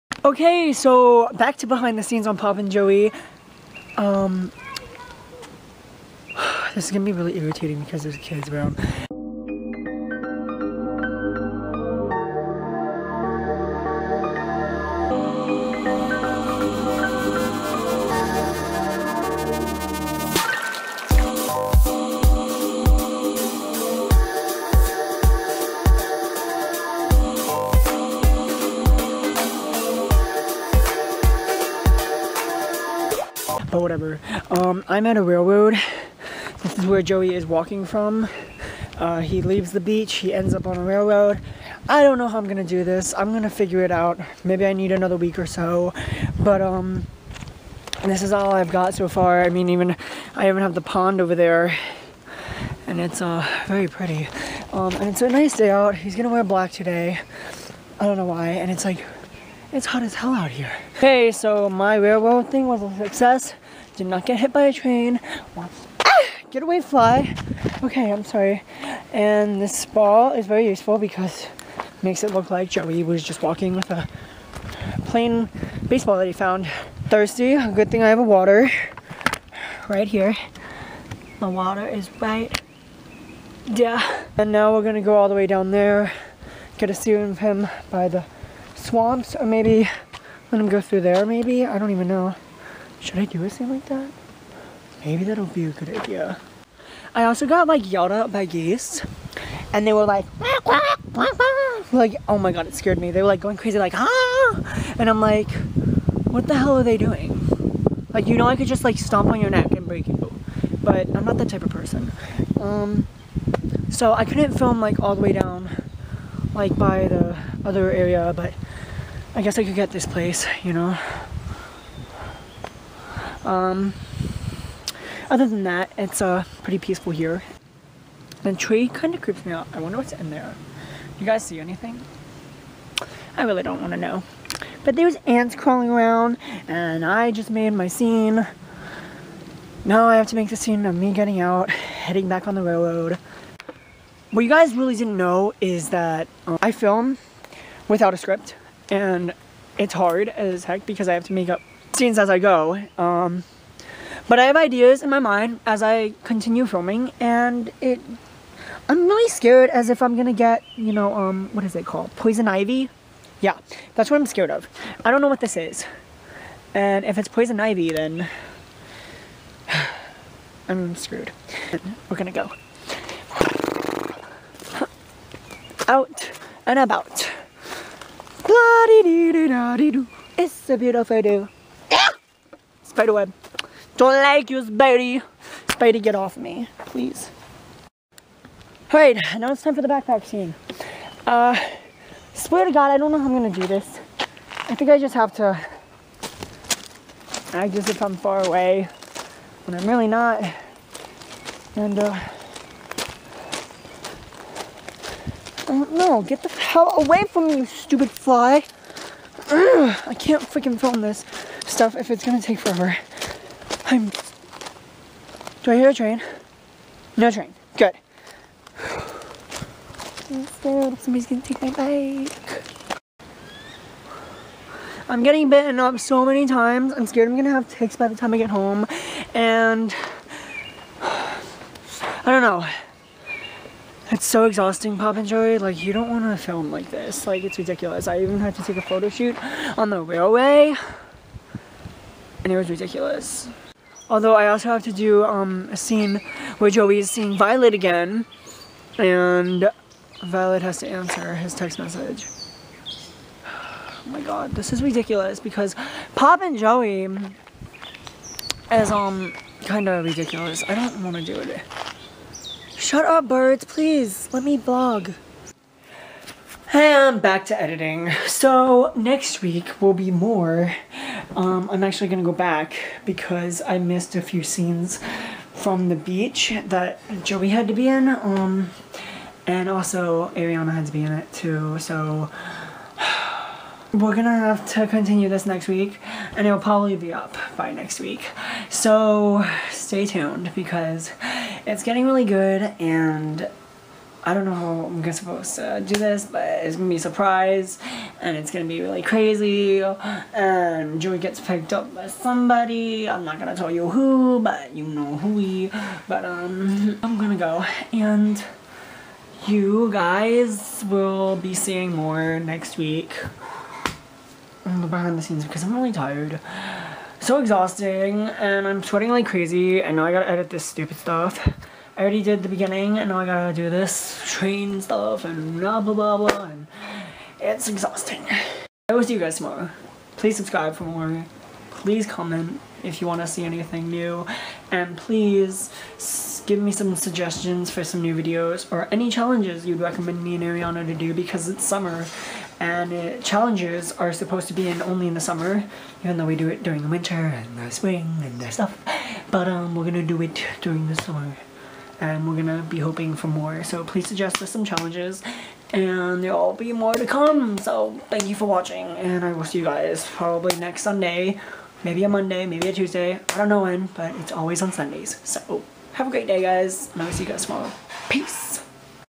MORE BEHIND THE SCENES OF POP & JOEY!
Another Day of Filming of Pop & Joey I ended up getting bit by bugs and almost had a heat stroke it was crazy today thank god I was not at a beach!

CAUTION!: WHATEVER I DID IN THIS VIDEO FOR FILMING DO NOT DO ON YOUR OWN!